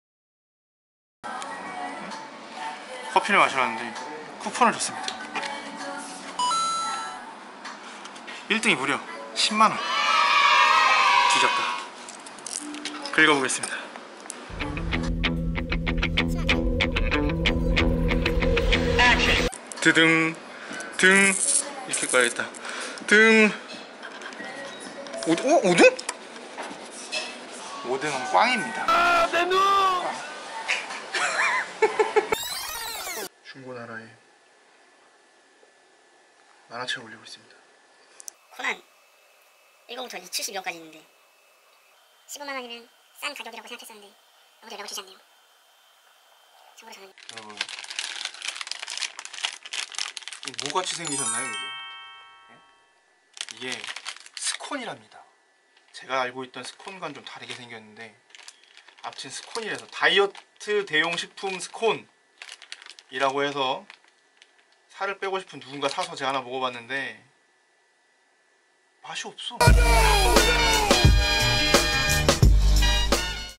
[VLOG] 작곡하는 아재의 우당탕탕 브이로그 #1
채널에 광고말고 뭔가 다른 컨텐츠도 넣어볼까 하다

브이로그를 올려보게 됐습니다.

뭐 별내용 없이 일상생활 그냥 이런저런 짧은 영상들입니다.
찍고보니 조금 욕심이 생기기도 하고.
어색한 모습들은 뭐 차차 나아지지 않을까 싶습니다

첫 영상이라 조금 어색해도 좋게 봐주셨으면 좋겠습니다 ㅋㅋㅋㅋ